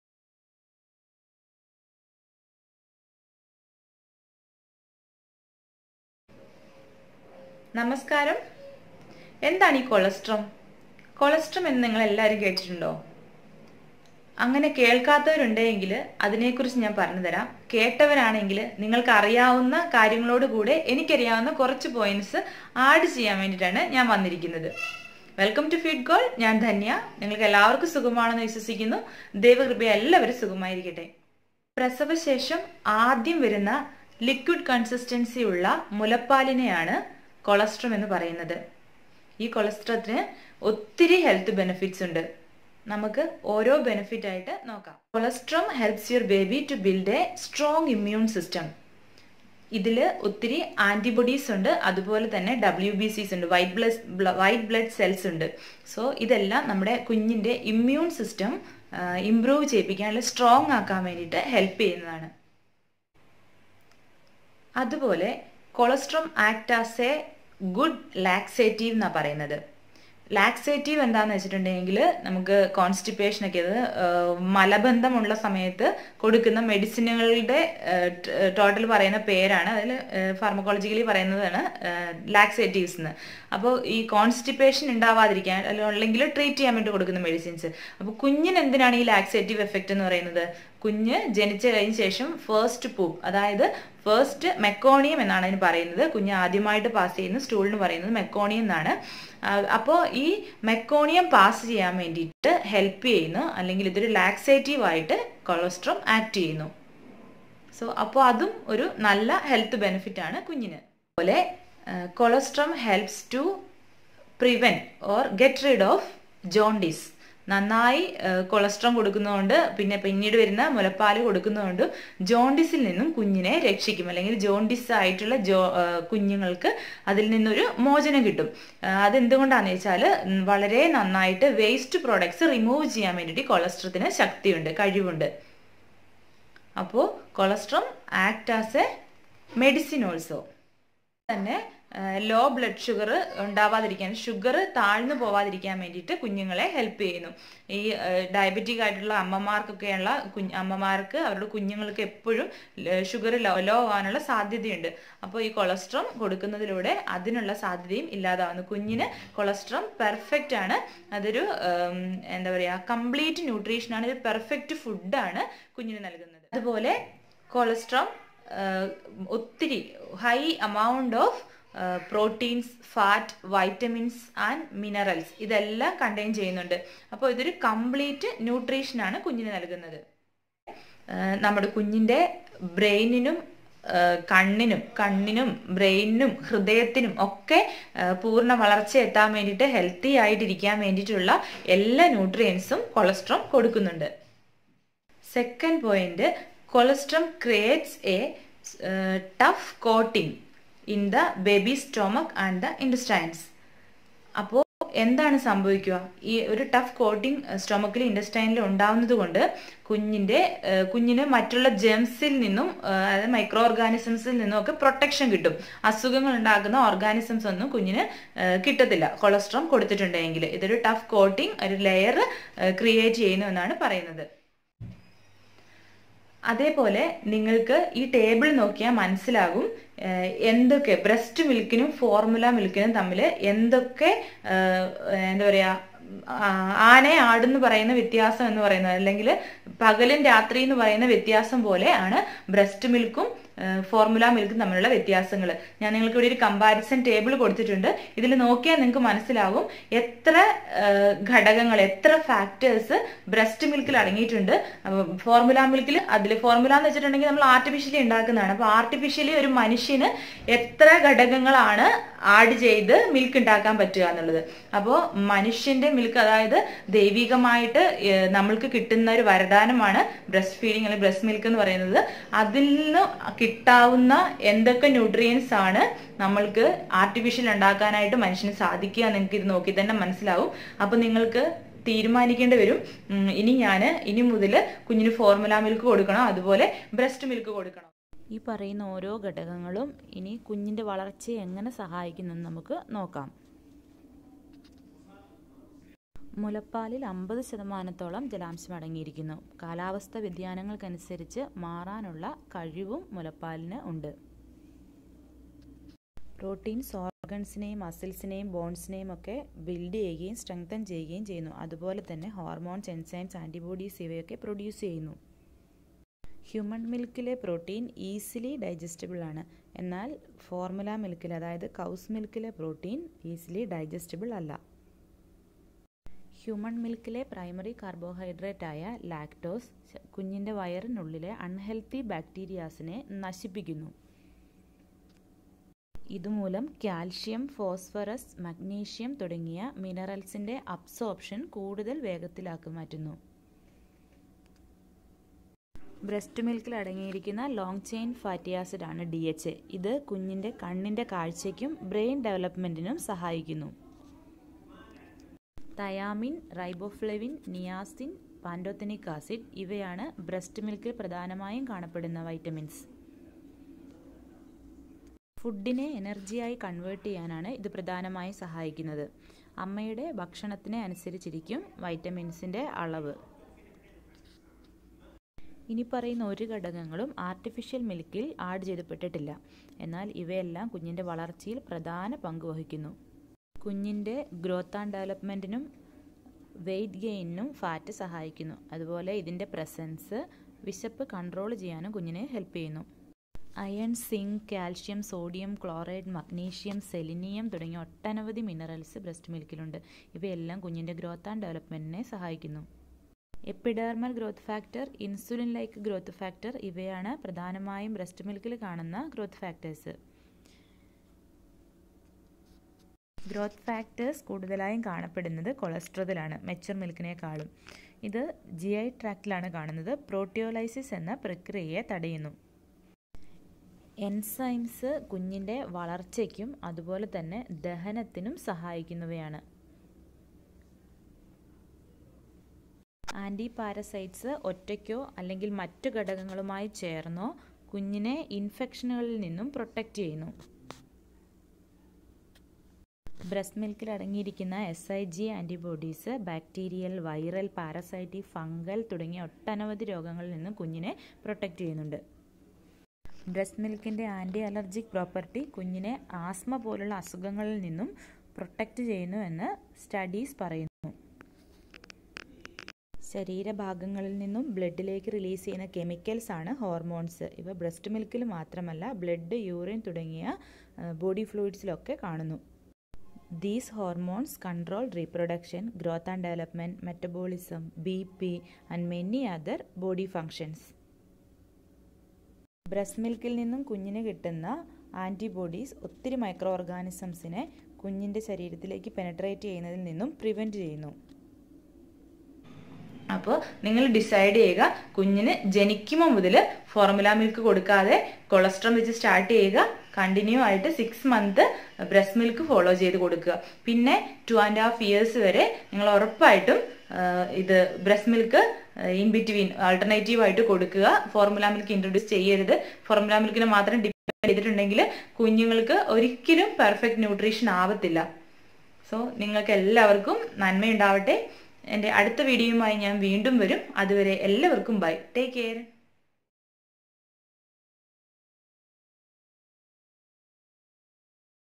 breastfeeding counselor എന്താണ് colostrum? Benefits of colostrum./feedgold
join @telegram  if you need any help regarding breast feed your baby.